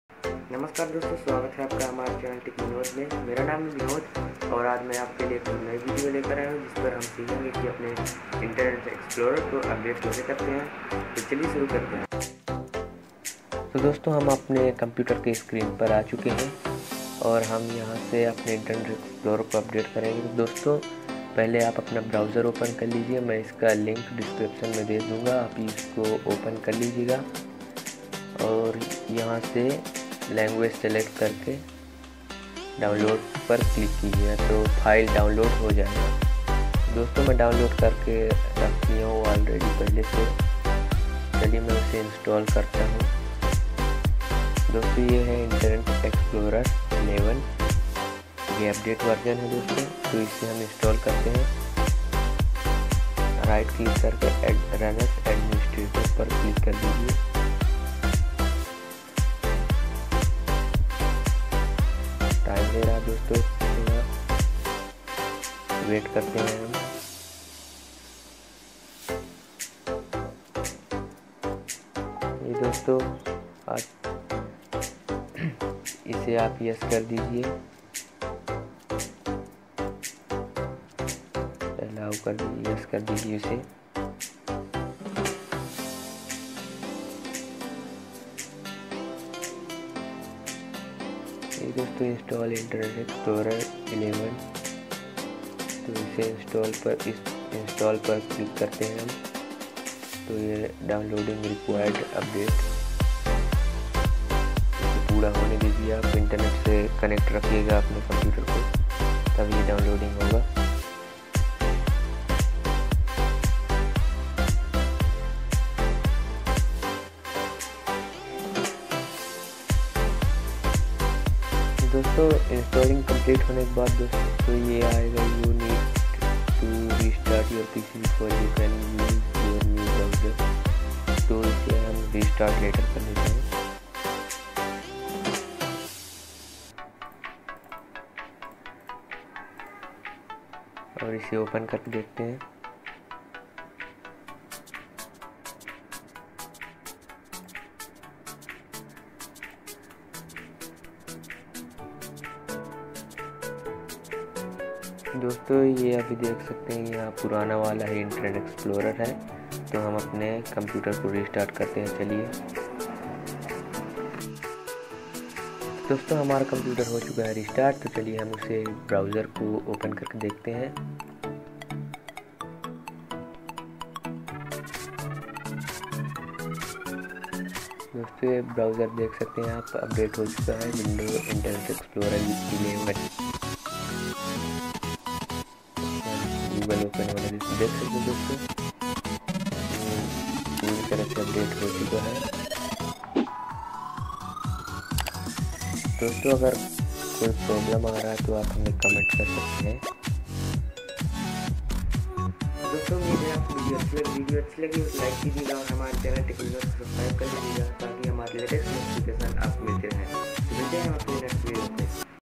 How to Update Internet Explorer In (Hindi)
Hello friends is video me maine aapko apne internet explorer ko update karne ki puri jankari hai ager apke browser out of date dikha raha hai to aapke liye ye video bahot hi badhiya sabit hoga